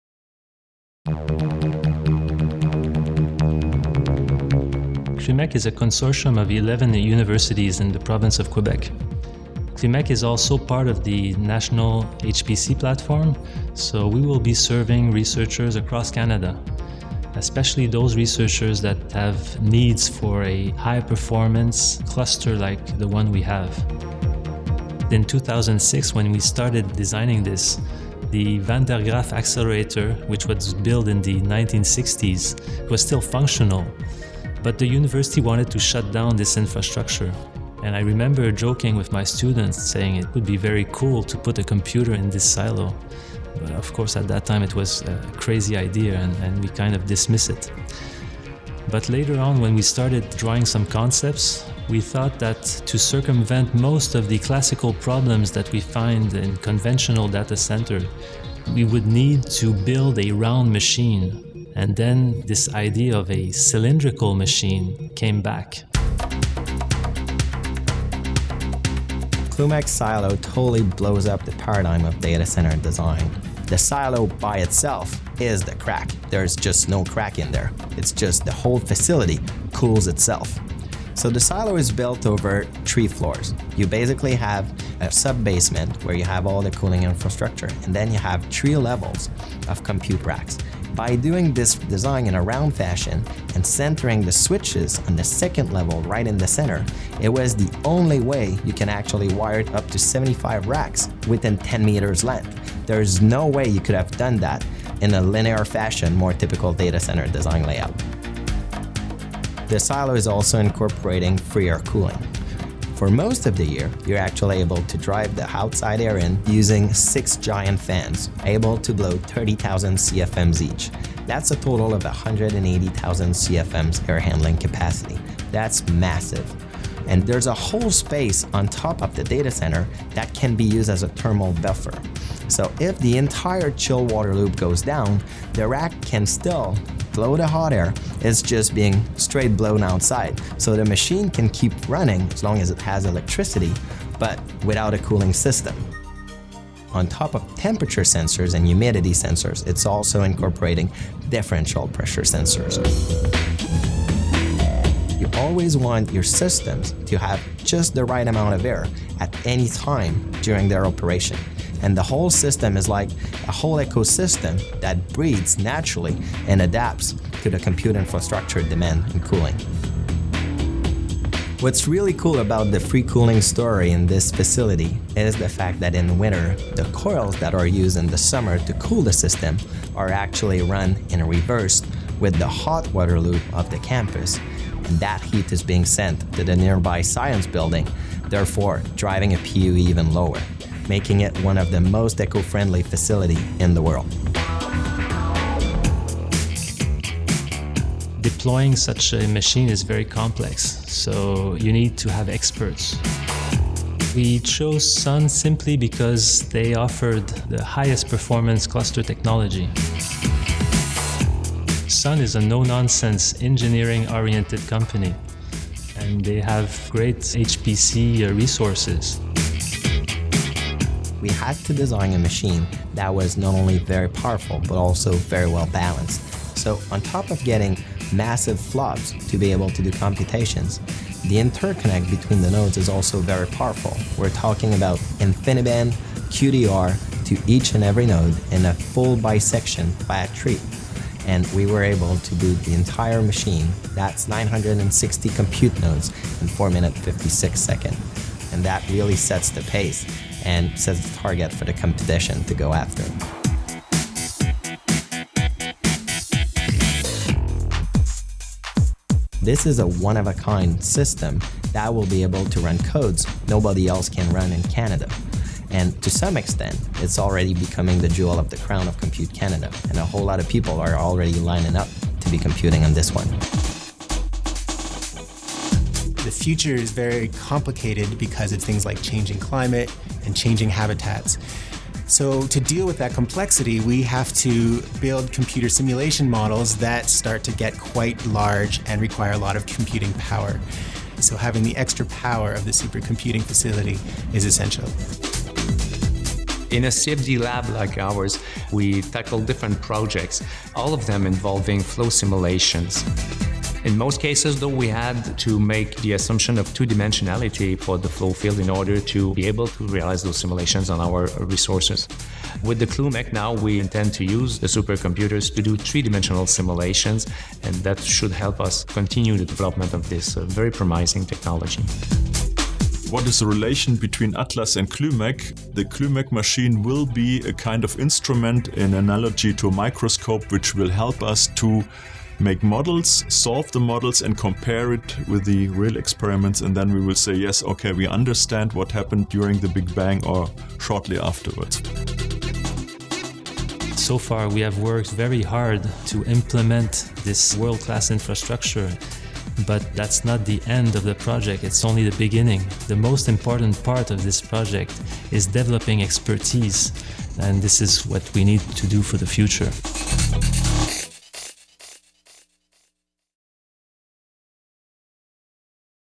Sun Constellation System CLUMEQ
How the CLUMEQ HPC consortium built one of the largest supercomputers in Canada working with Sun.